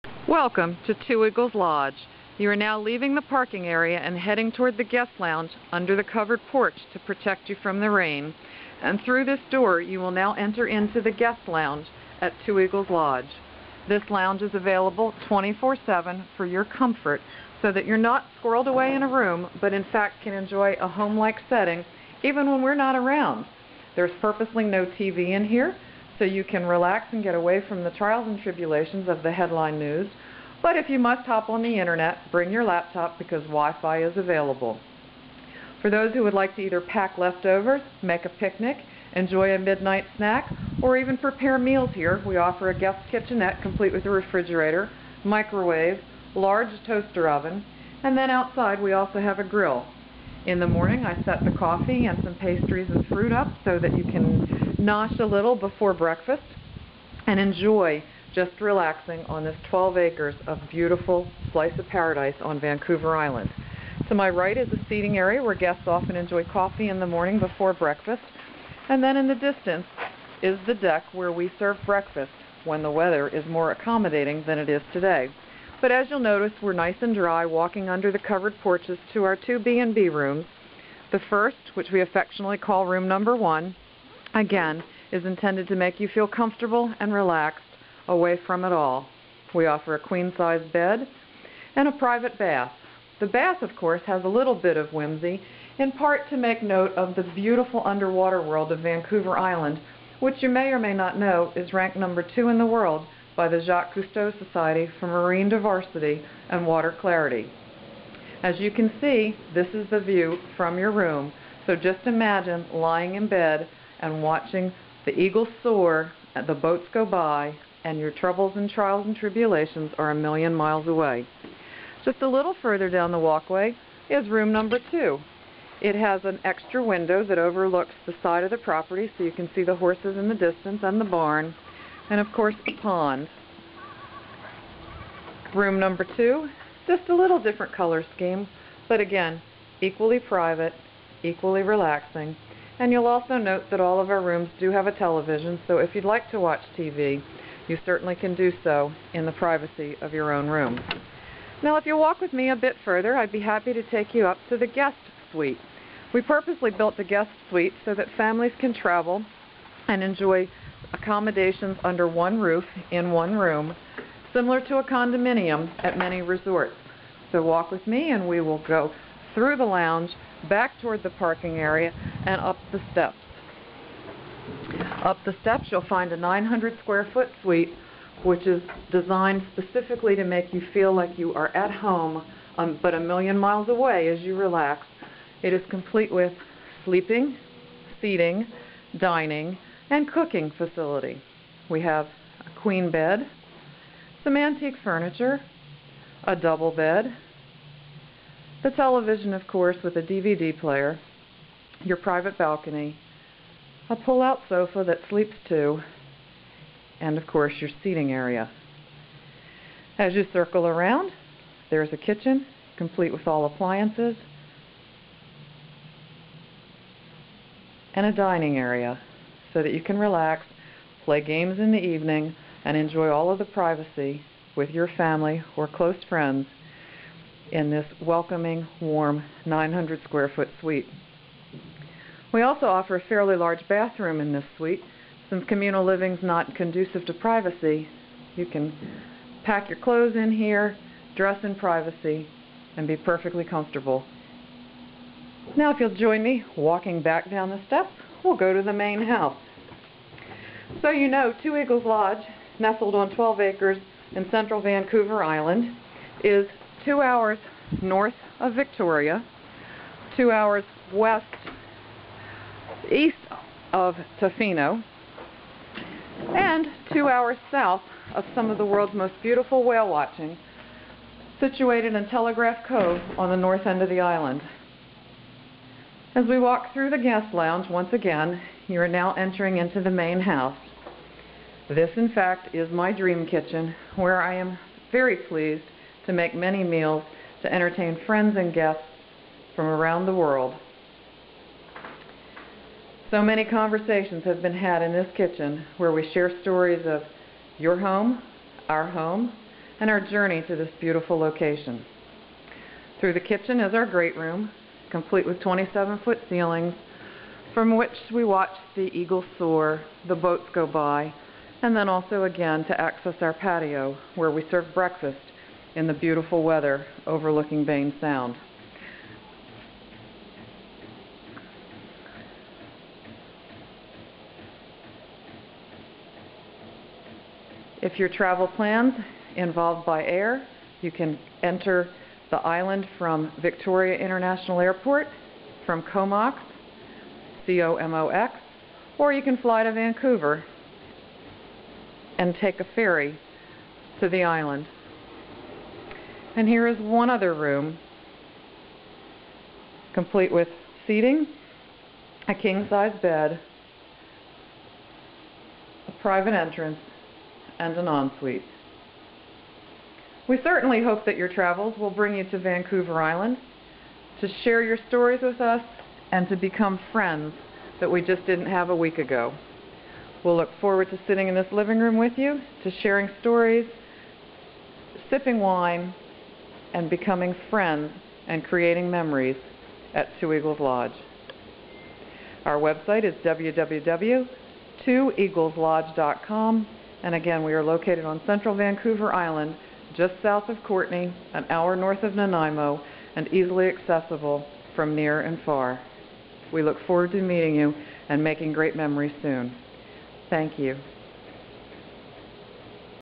Two Eagles Lodge Union Bay, British Columbia Canada
2011 Tourism/Hospitality Business of the Year for Vancouver Island!  Waterfront B&B on 12 pastoral acres of Vancouver Island's east coast was built in 2007, offering spectacular views of the water and coastal mountains from every room.   Guests enjoy private entrances and private baths, a guest lounge and kitchenette (open 24/7).  A vacation suite is perfect for families!  Sit on the porch and watch the horses play and the eagles dive for fish...or tour all there is to do on this beautiful island from our central location (just 2 hours to Victoria, Tofino, and the world's best whale watching.  Hosts Carolyn & Steve "discovered" this island in '04 and love to share it with others.  See TripAdvisor reviews, which "preview" what you'll experience...then pack your bags for a vacation you'll never forget.